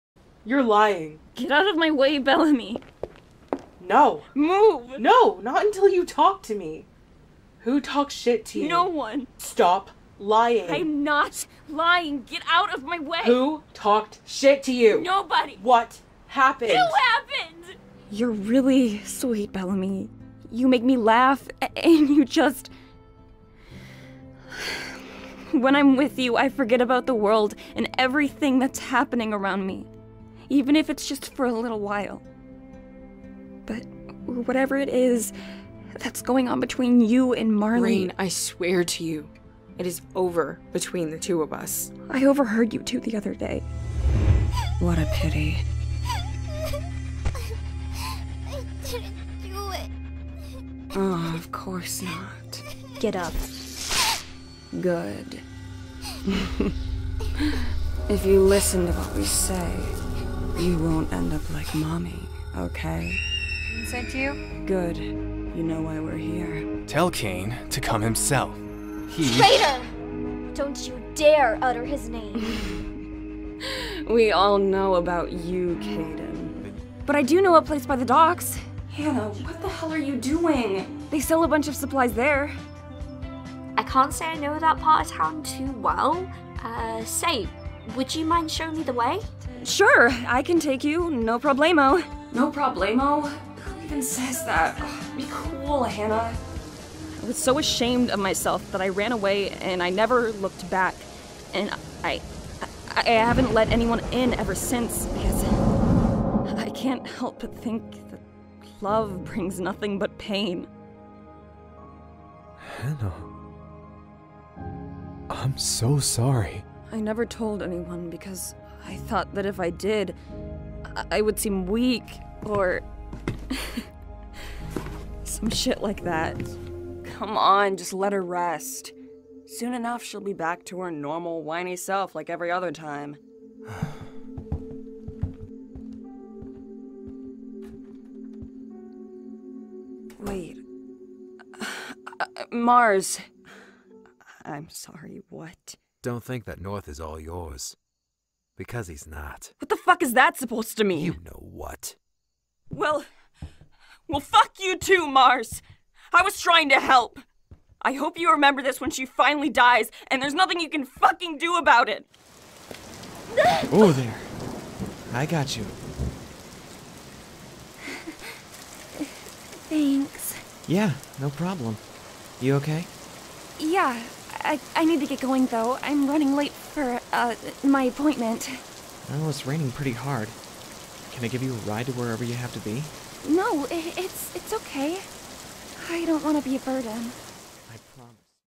Machinima Voice Reel: June 2018 - September 2019
I've wanted to make one of these for a bit now, and I just realized that the only projects I've voiced in that have actually come to fruition in 2019 so far are Sims series (and fandubs which I chose not to include)... so here I am! I would love to branch out from Sims into projects like visual novels, animations, and more. I have a few great roles coming up that I can't talk about yet due to nondisclosure agreements, but I'm super excited!

I'm currently only taking on paid roles or roles I'm super interested in. I'm especially interested in science fiction, fantasy, and anything LGBTQ+.